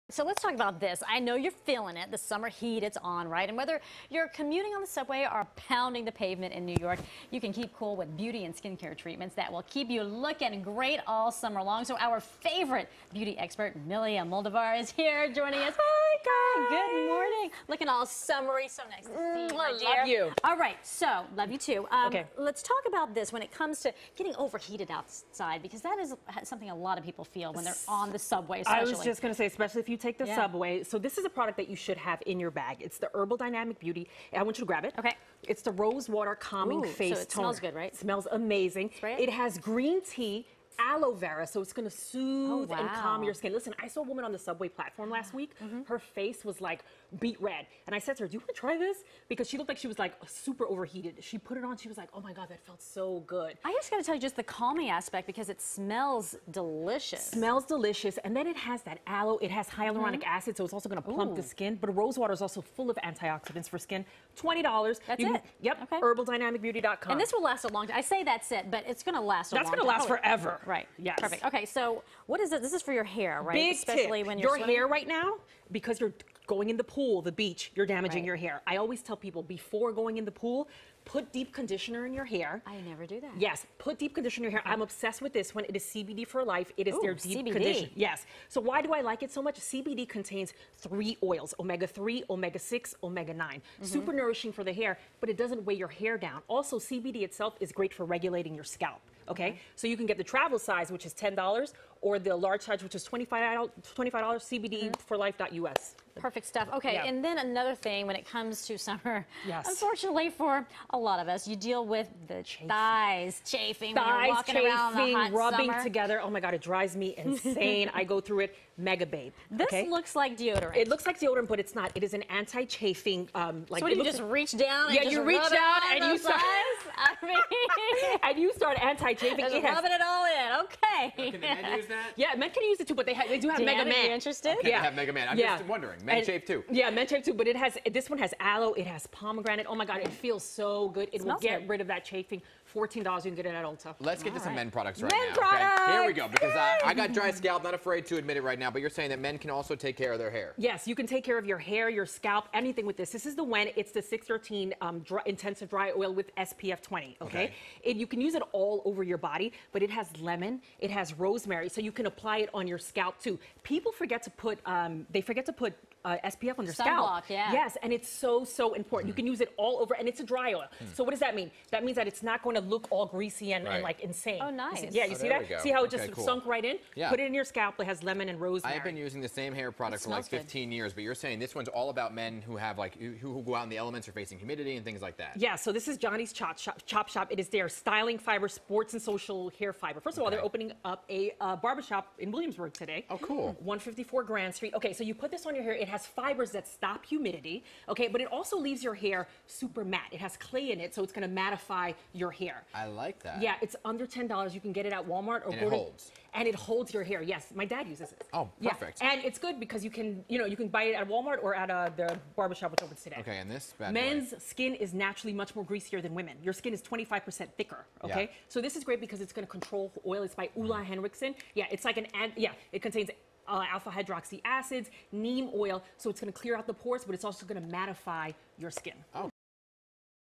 Summer beauty and skin treatments
The summer heat is on! Whether you're commuting on the subway or pounding the pavement in New York, you can keep cool with beauty and skincare treatments that'll keep you looking great. Beauty expert Milly Almodovar shares several summer beauty fixes for both men and women.